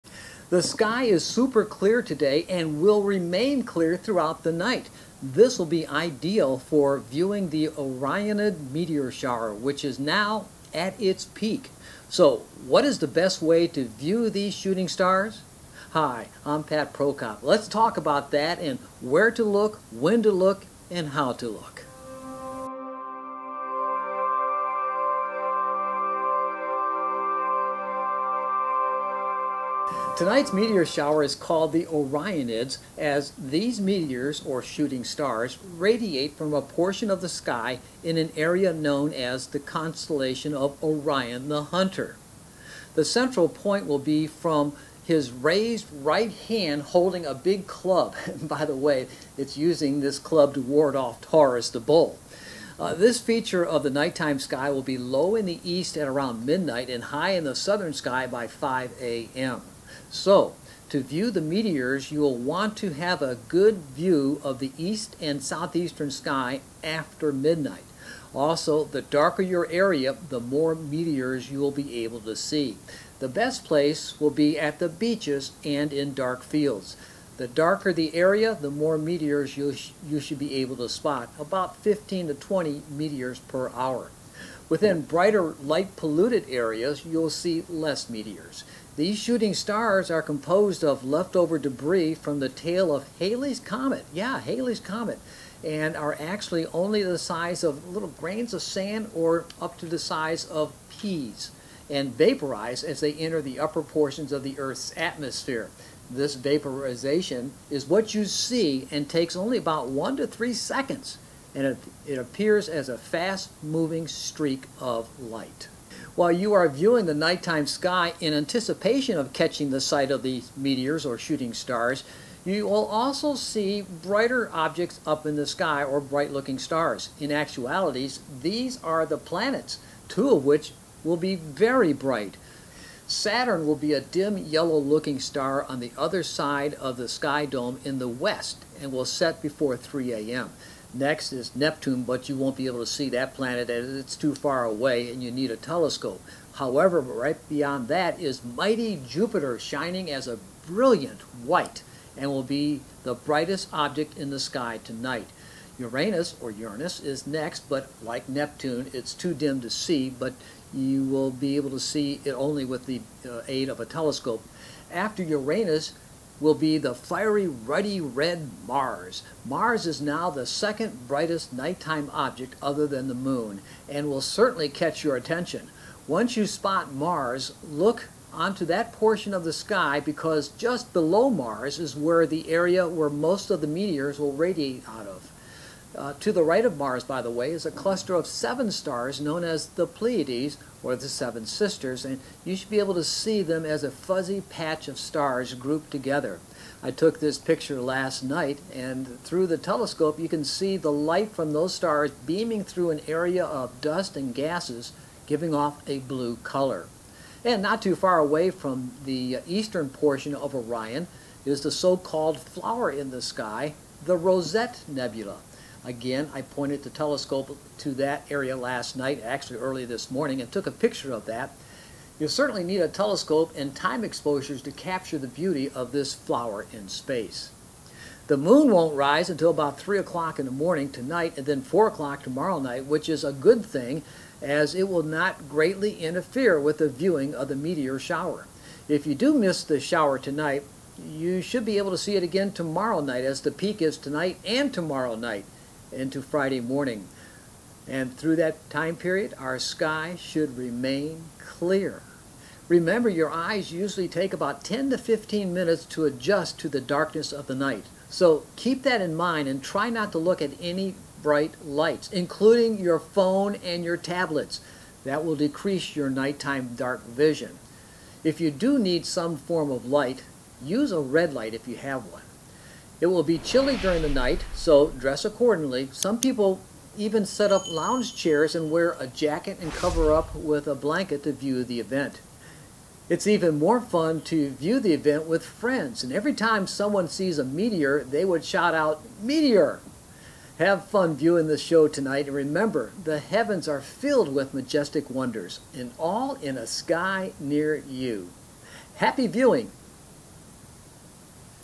Time to View the Orionids Meteor Shower
It's that time for the Orionid Meteor Shower and as luck would have it, we have extremely clear skies and the moon won't interfere with the viewing.  I will show you WHERE to look, WHEN to look, HOW to look, and what these meteors are made of.